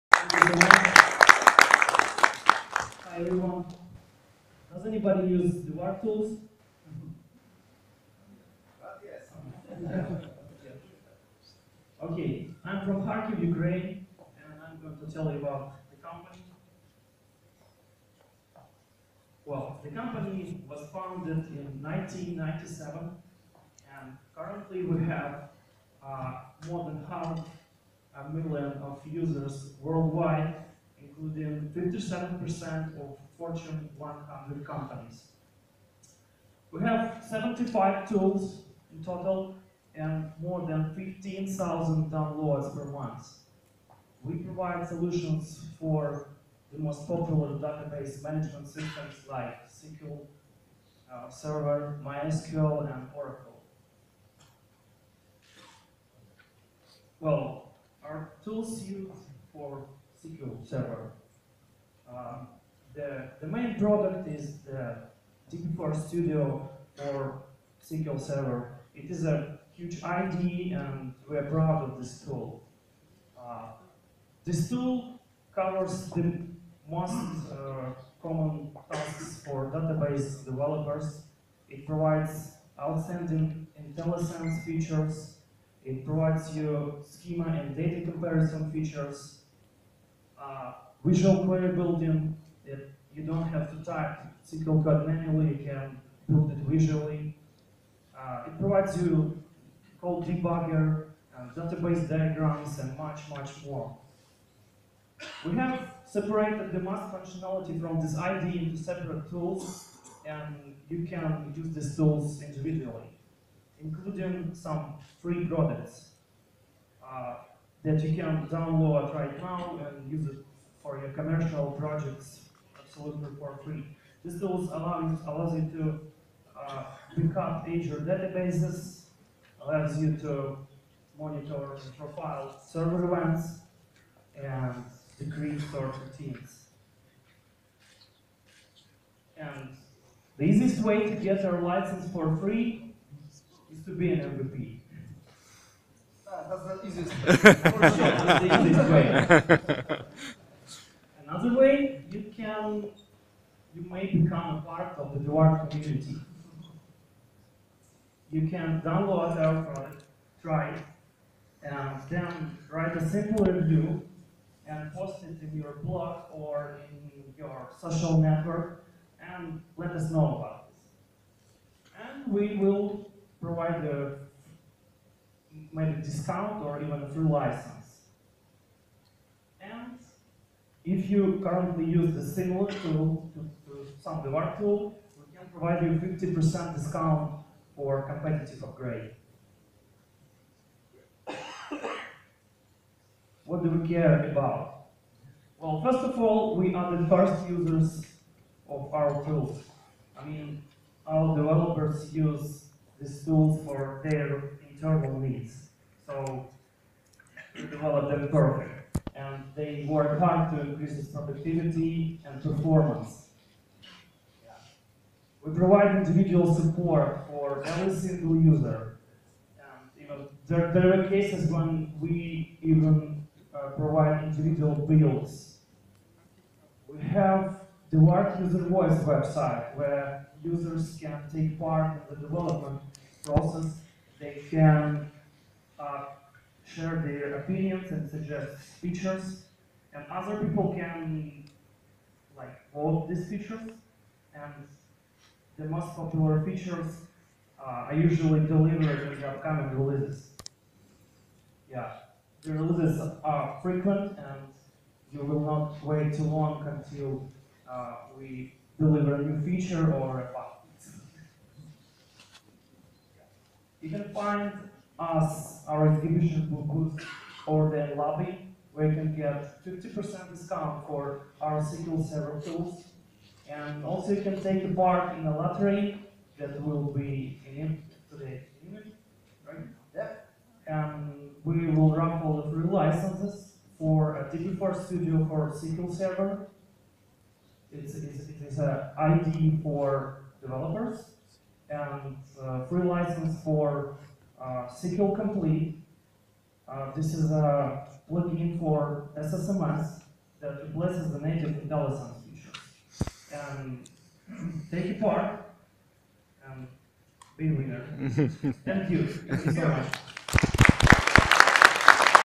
Presenting Devart company at SQL Day 2015 | Annual Conference of Polish SQL Server User Group
Devart was the GOLD sponsor of the SQL Day | Annual Conference of Polish SQL Server User Group, which was held on the 11th - 13th may 2015 in Wroclaw.  We have had a great chance to meet new people, exchange views, and present our software products to the wide audience.